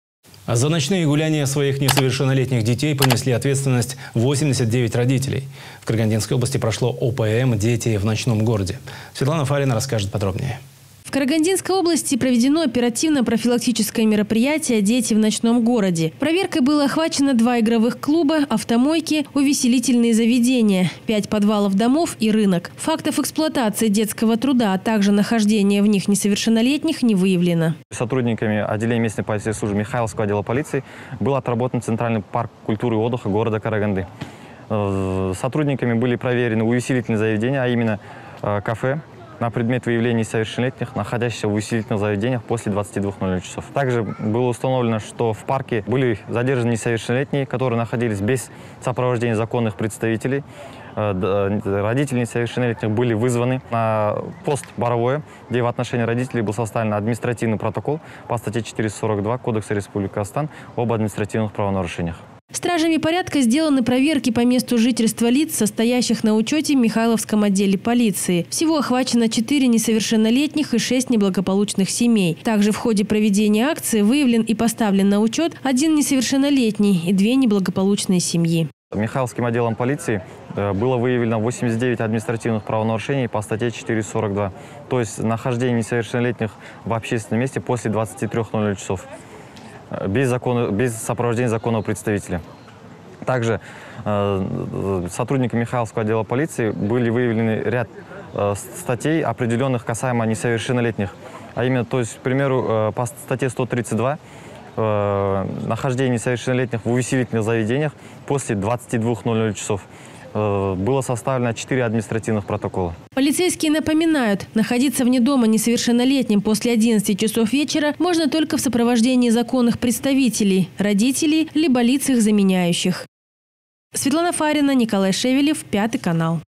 За ночные гуляния своих несовершеннолетних детей понесли ответственности 89 родителей
В Карагандинской области прошло ОПМ "Дети в ночном городе"